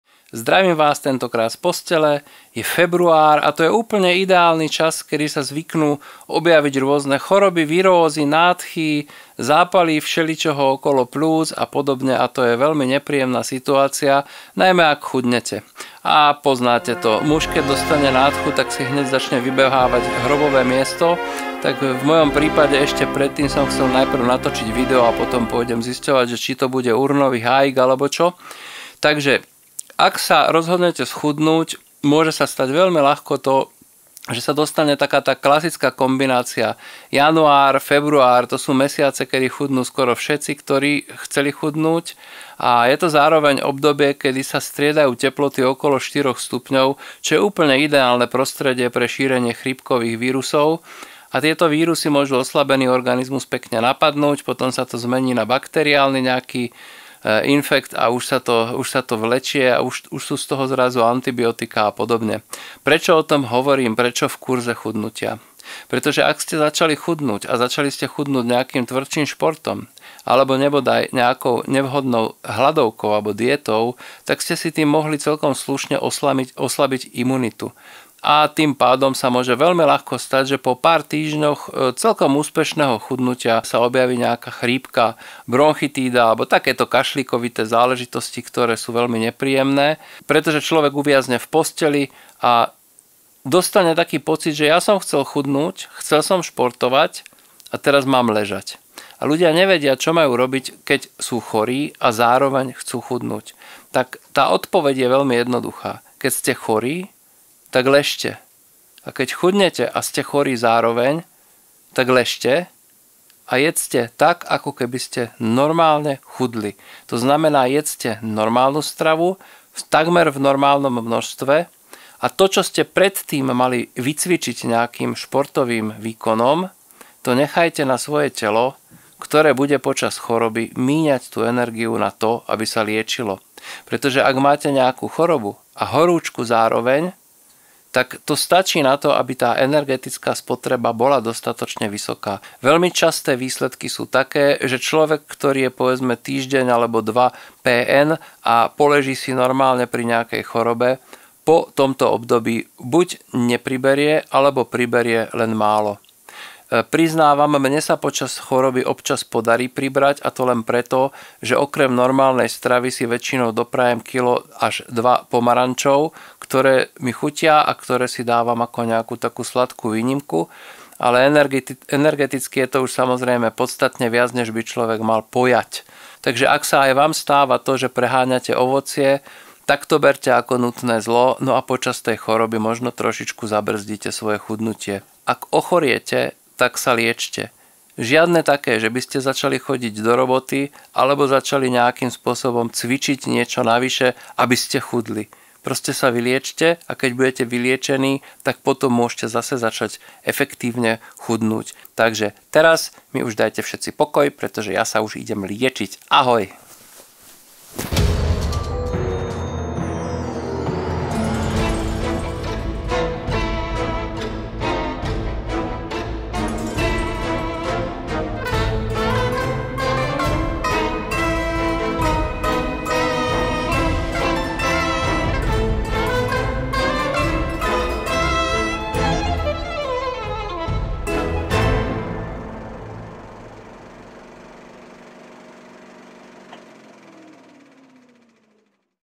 Choroba a chudnutie - ako zmeniť životosprávu počas choroby? | Kurz chudnutia, 11
Január a chrípka sa snúbia. Najmä muž ak má nádchu tak už objednáva hrobové miesto
Ľudia začnú chudnúť a ochorejú.

Prepísknutý tréning.
Podvýživa.

Ideálne počasie na šírenie infekcií začiatkom roka. Oslabenie a ochorieme.

Ak chudnete nič nemeňte.
Jedzte zdravo.
Choroba nahradí tréning.
Horúčka je spotreba energie. Vy ležíte ale telo pracuje.
V žiadnom prípade necvičte.
Najlepšie je nechodiť ani do práce ak sa dá.
Potrebujete odpočinok.
Môže byť že trocha priberiete, mne sa to stáva ak zjem plus kilo pomarančov.

Ak máte otázky, pýtajte sa v komentároch k videu.

Hudba je zo zvukovej knižnice ak si ju objednáte z tohto linku dostanete zadarmo 2 mesiace navyše a ja tiež.